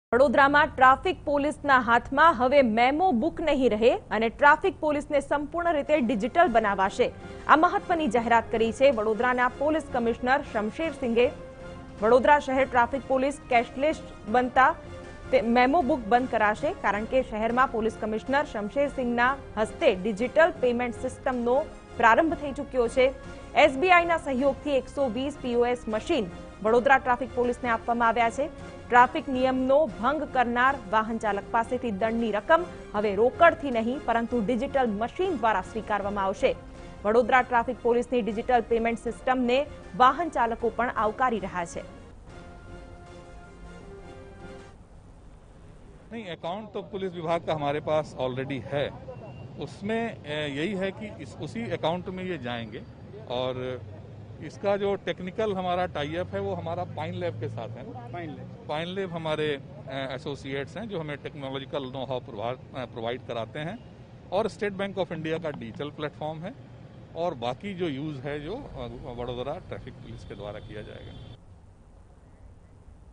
Vadodara traffic police goes digital, launches digital memo system for traffic violators| TV9News
Vadodara traffic police goes digital, launches digital memo system for traffic violators| N09